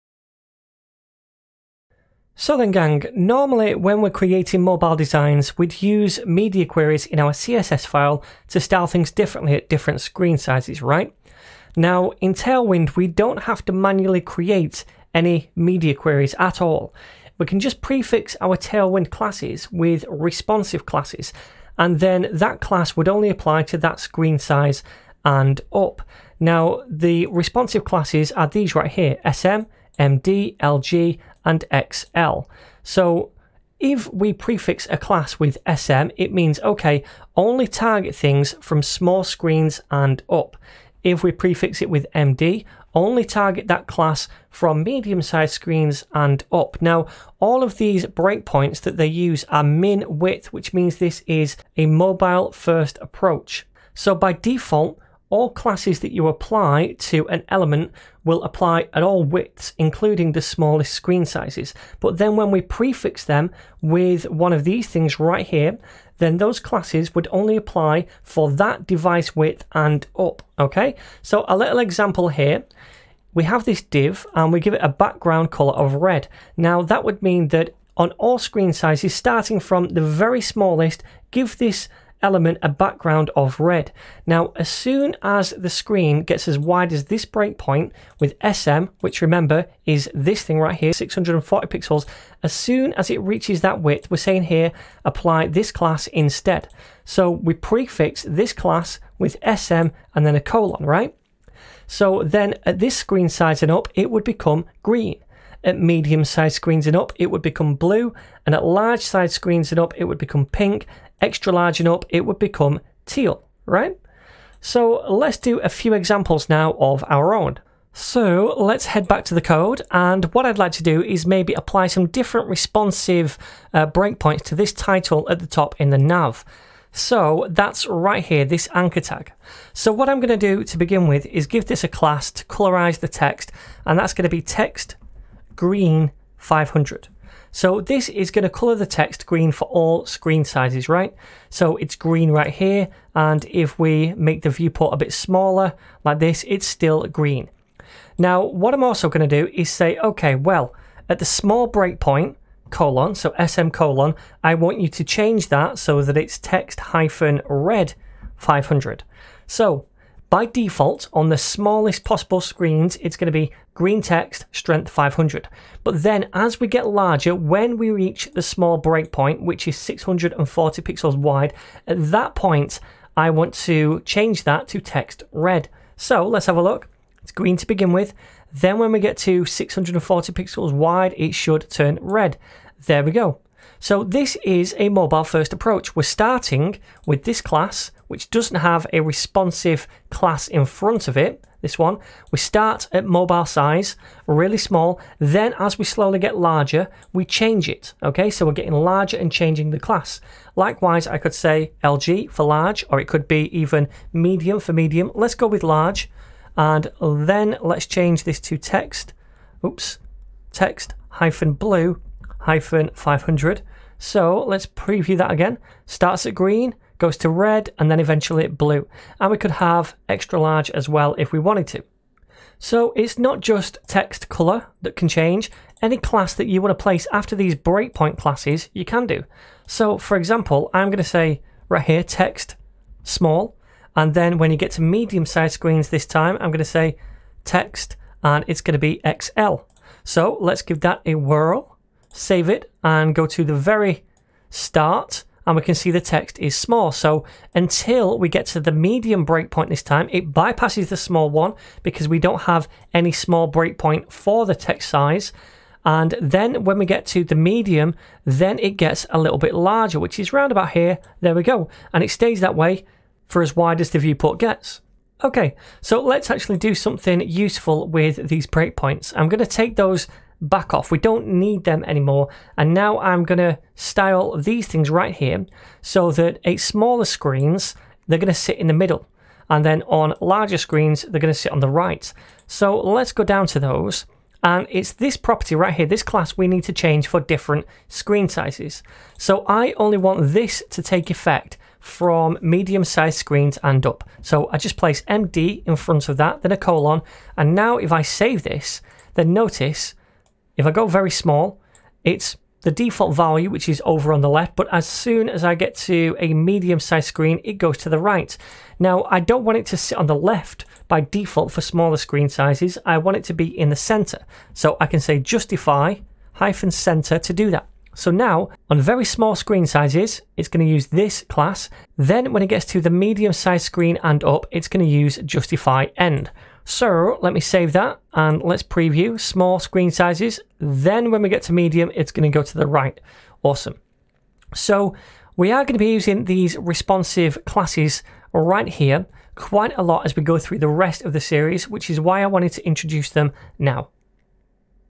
Tailwind CSS Tutorial #8 - Responsive Classes
Hey gang, in this tailwind css tutorial we'll have a look at some of tailwind's responsive classes to create mobile / tablet styles.